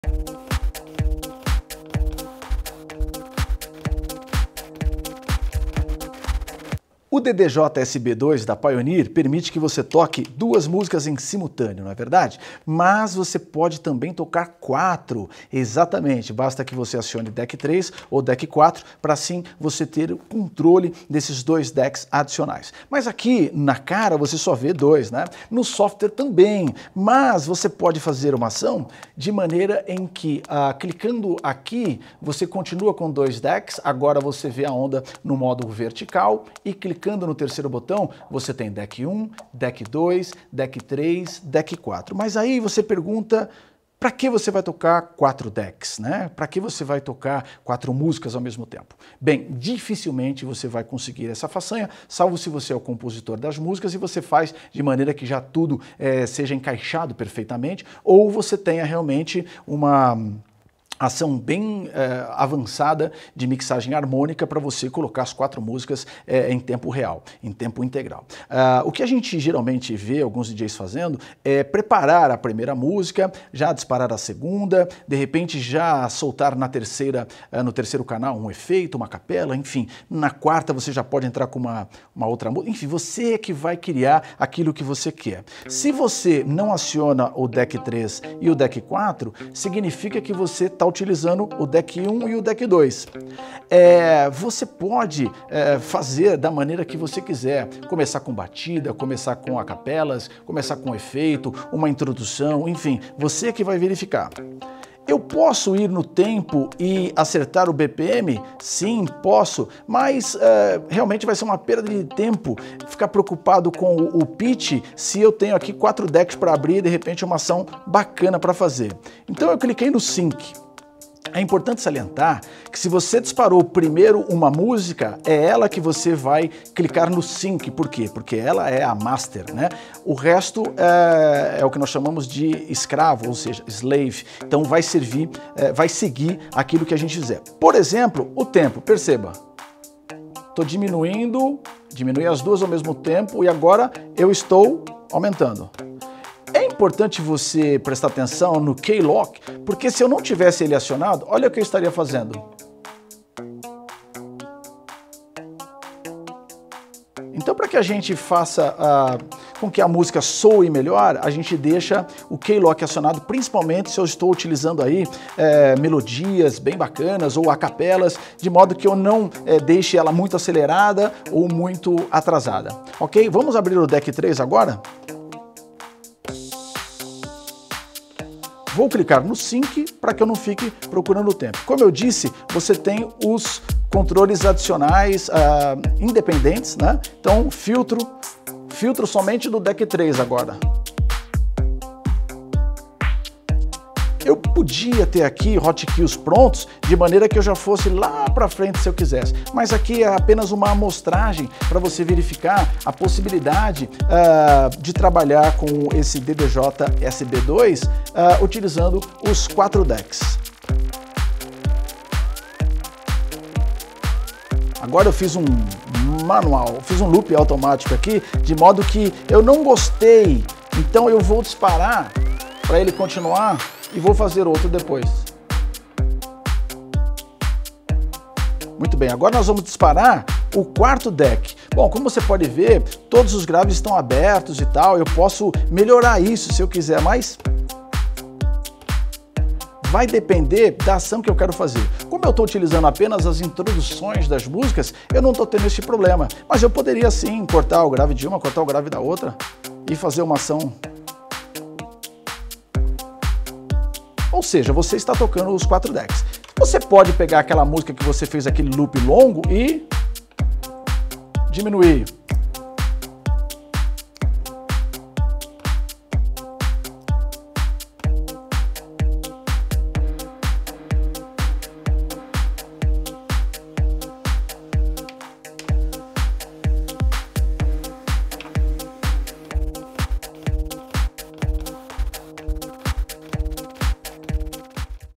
DDJ-SB2: 2 ou 4 CANAIS EM SIMULTANEO - Pioneer DJ (Vídeo 4 de 5)
Pioneer DDJ-SB2 - Controlador 4 decks com Serato Intro

Confira as séries de vídeos Pioneer DJ

O DDJ-SB2 leva você além do controle. O novo modelo DDJ-SB2 não só apresenta todas as características populares do DDJ-SB, como também adiciona algumas novas funções "pro-DJ" surpreendentes.

O controlador foi atualizado para 4 decks e botões dedicados para alternar entre os canais sem esforço. Além disso, foi adicionado um botão de ajuste de ganho de entrada por canal (trim), com meter grafico mostrador de sinal tb.

Conteúdo da embalagem:

Pioneer DDJ-SB2
Cabo USB
Manual do usuário.

Plug e Play

Serado DJ Intro (Download Gratuito)

Serato DJ (Upgrade Pago)

 Requerimentos de Sistema

Windows 8.1/Windows 8/Windows 7 with Service Pack 1

• CPU: Intel 2.0 GHz Core Duo/Intel i3, i5, and i7 Core
• Memory: 1GB

Mac OS X 10.10/10.9/10.8

• CPU: Intel 1.6 GHz Core Duo/Intel i3, i5, and i7 Core
• Memory: 1GB

Display Resolution
1024 x 768 or higher